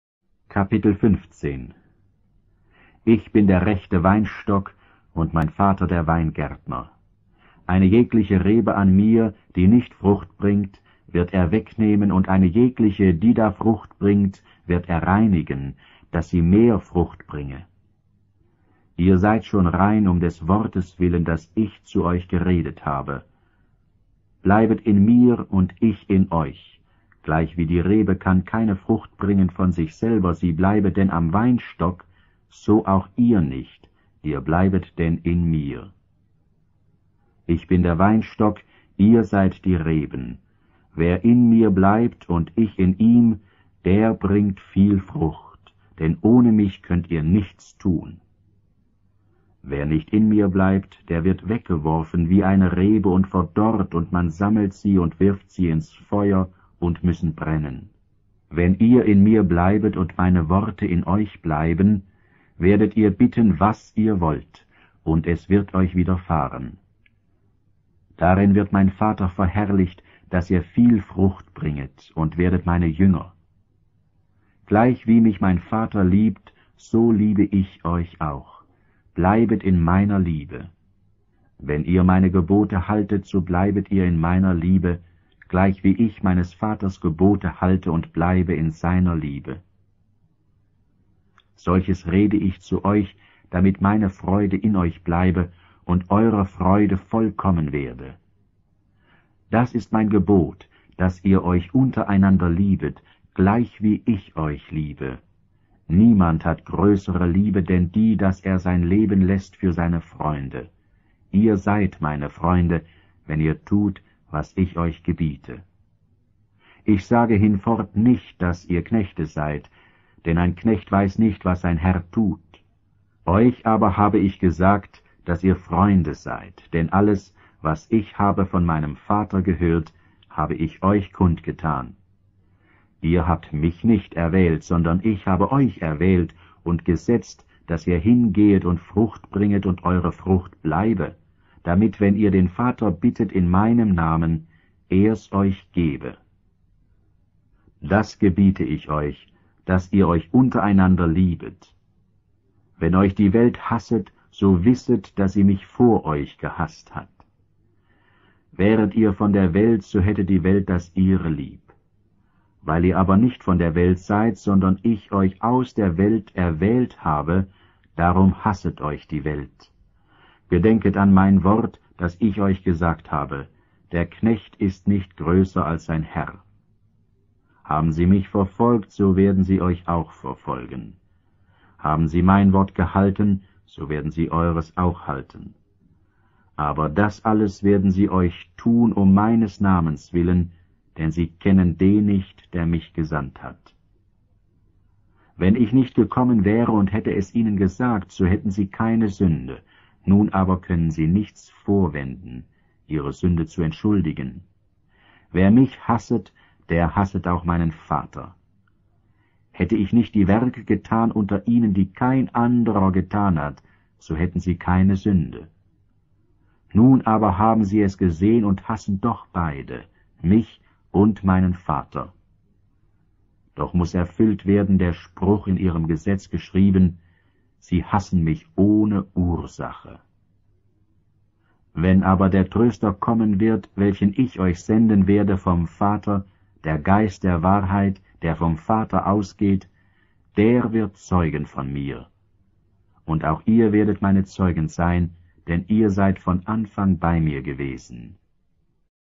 Das Evangelium nach Johannes - Kapitel 15 [LuÜ]
Titel: Das Evangelium nach Johannes - Kapitel 15
Autor: Audio-Bibel (Luther Übersetzung NT 1956/AT 1964)
Bibelstelle: Johannes 15,1-27
Sprache: deutsch
Kategorie:  Audio-Bibel, Hörbibel, Bibeltext gelesen, Audiobibel